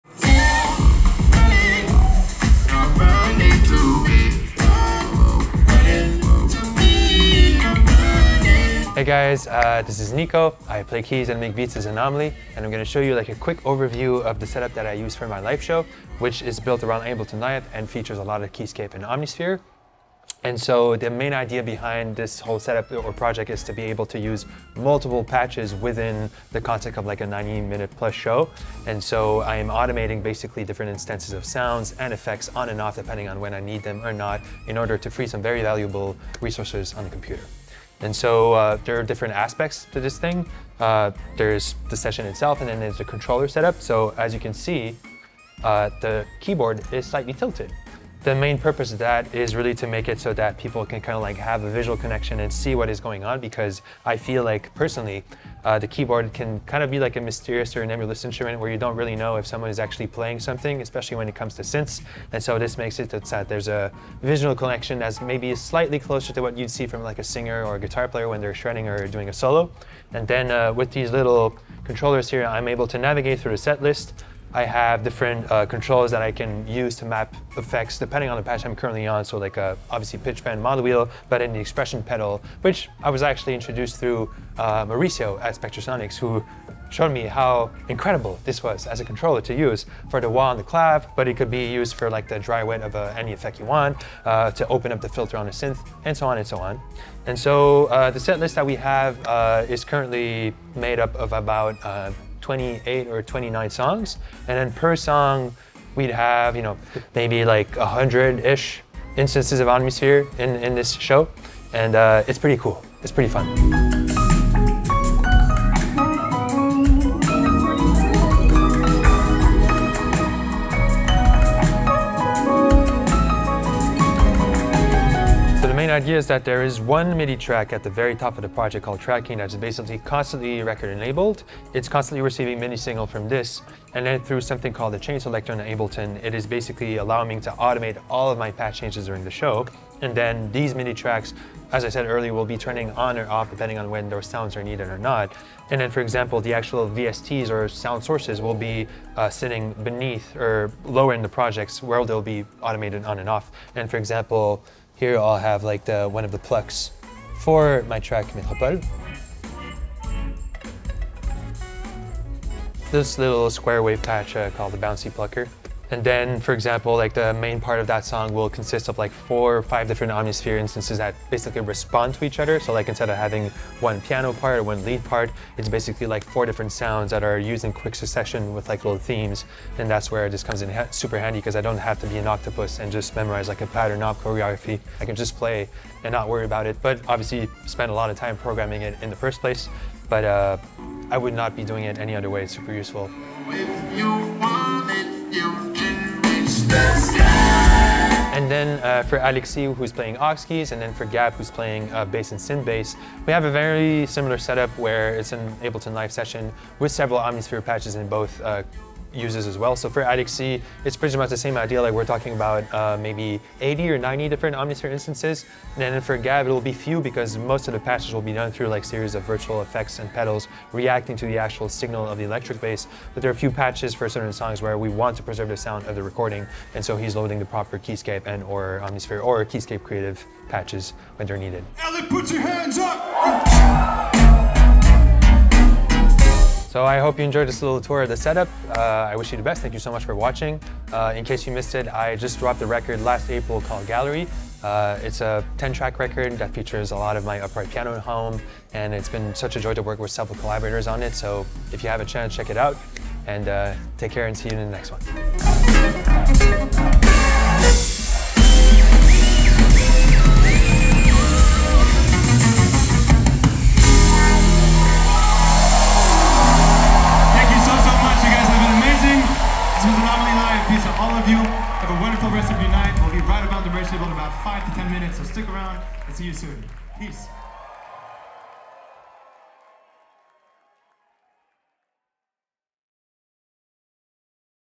100 OMNISPHERES! - ANOMALIE live rig
Our good friend Anomalie gives us a behind the scenes look at his incredibly innovative live rig, featuring 100 instances of Omnisphere! We caught up with him and his band on the road at the El Rey Theater in Los Angeles.

Anomalie's Computer Specs: M1 Max 16inch Macbook Pro, 10-Core CPU, 64GB RAM, 4TB SSD

He is using a Roland A-88 to play the Spectrasonics instruments in this video. The keyboard stand is a custom Gibralter build.